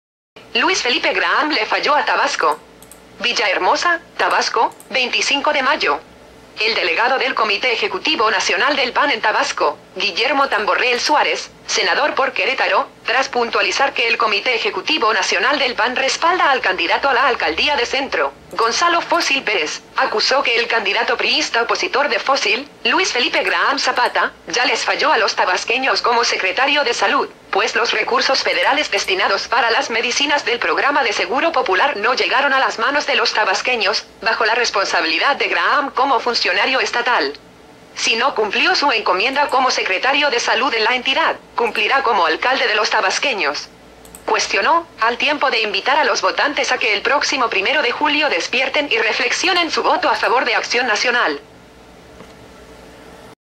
Luis Felipe Graham le falló a Tabasco.mpg
Luis Felipe Graham le falló a Tabasco.
Villahermosa, Tabasco, 25 de mayo.
El delegado del Comité Ejecutivo Nacional del PAN en Tabasco, Guillermo Tamborrel Suárez, Senador por Querétaro, tras puntualizar que el Comité Ejecutivo Nacional del PAN respalda al candidato a la alcaldía de Centro, Gonzalo Fócil Pérez, acusó que el candidato priísta opositor de Fócil, Luis Felipe Graham Zapata, "ya les falló a los tabasqueños como secretario de Salud, pues los recursos federales destinados para las medicinas del programa de Seguro Popular no llegaron a las manos de los tabasqueños, bajo la responsabilidad de Graham como funcionario estatal".
Si no cumplió su encomienda como secretario de Salud en la entidad, ¿cumplirá como alcalde de los tabasqueños?, cuestionó, al tiempo de invitar a los votantes a que el próximo primero de julio despierten y reflexionen su voto a favor de Acción Nacional.